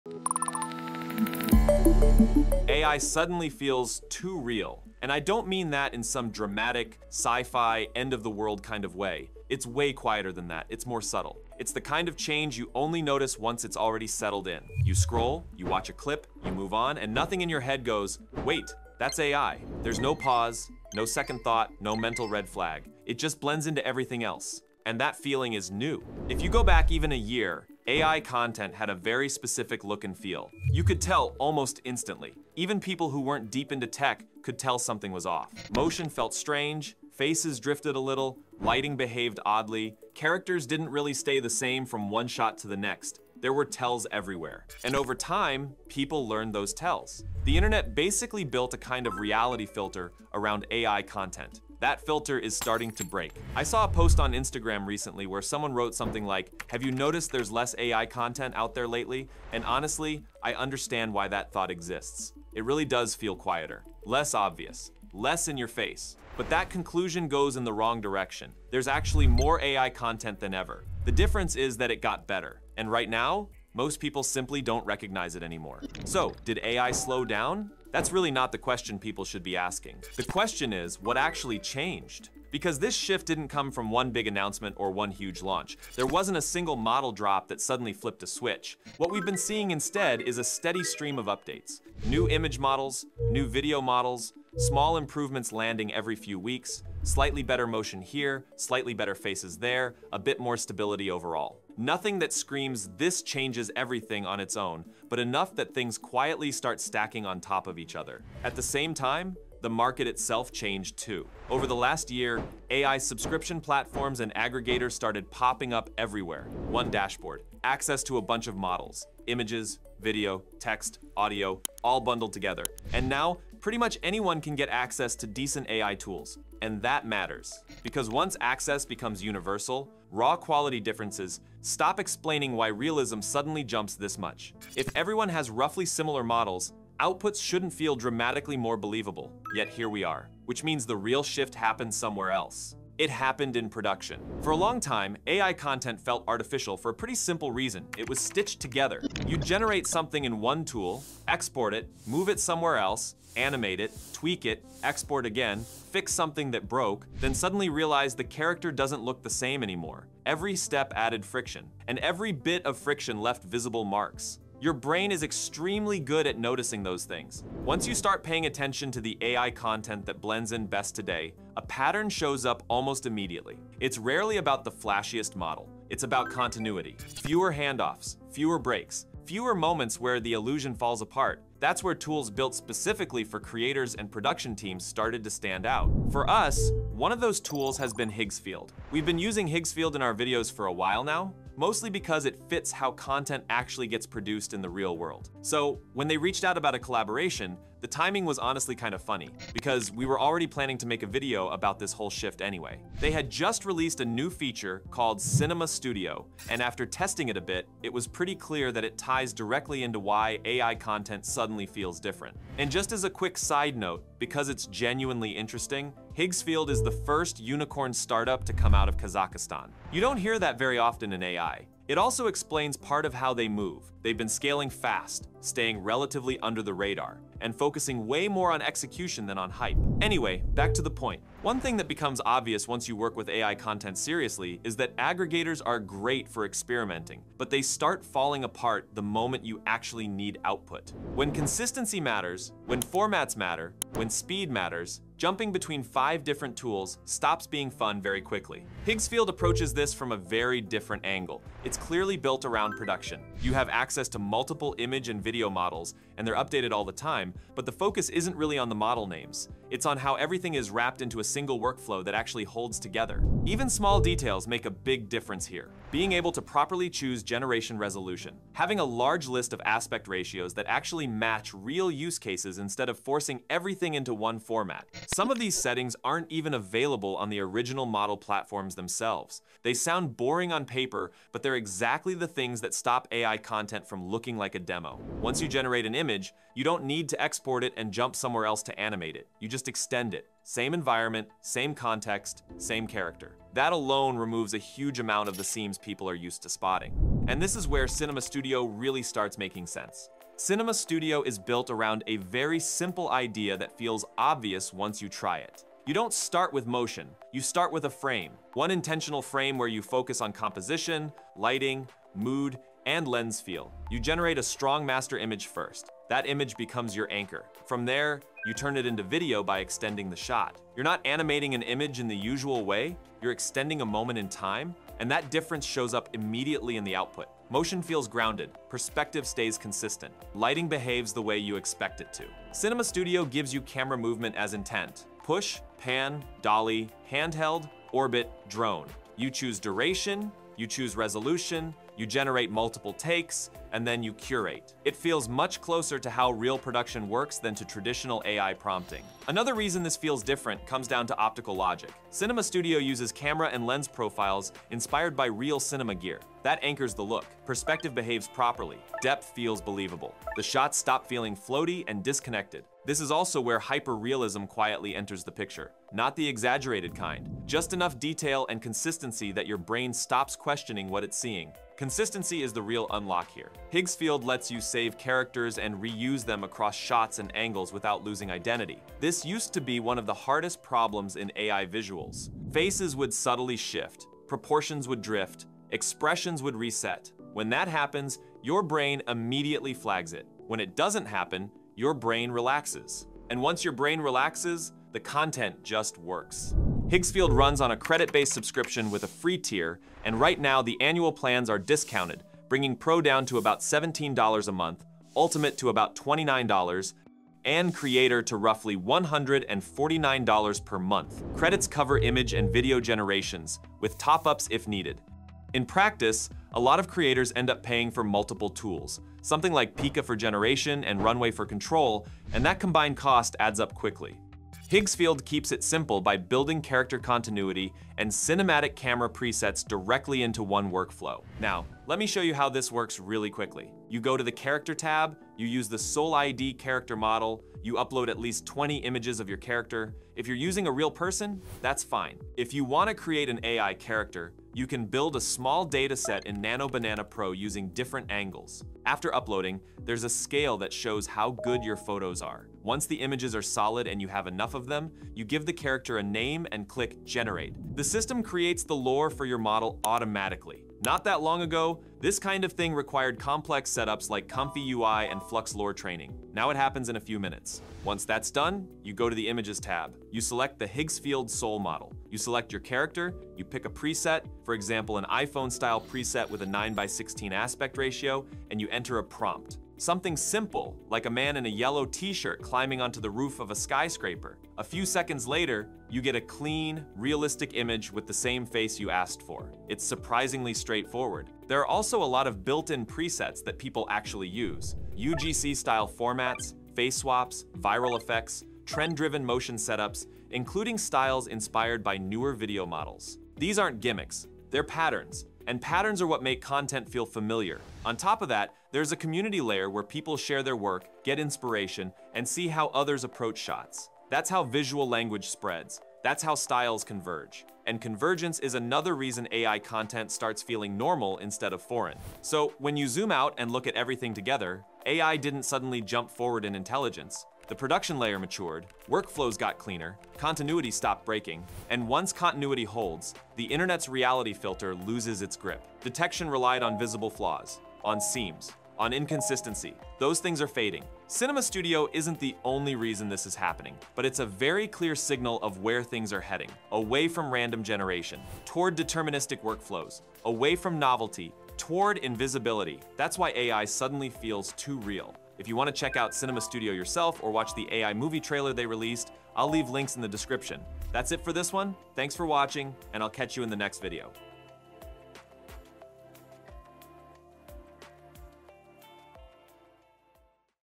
AI Broke the Reality Filter and Its Freaking People Out
Something quietly changed over the last year. AI content stopped announcing itself. It stopped feeling stitched together. Motion holds. Faces stay consistent. Lighting behaves. And most people don’t even realize they’re watching AI anymore.
This video breaks down what actually changed — and it’s not one model release or some sudden intelligence jump. The real shift happened in production: cleaner workflows, fewer handoffs, and continuity that finally holds. Tools like Higgsfield and its new Cinema Studio make that shift obvious once you see how they work.

- Why AI suddenly feels different
- Why it’s not about one big model drop
- The collapse of obvious AI “tells”
- Aggregators vs real production workflows
- Higgsfield overview and Cinema Studio
- Character continuity and why realism sticks

AI realism didn’t jump because models got smarter overnight.
It jumped because continuity stopped breaking.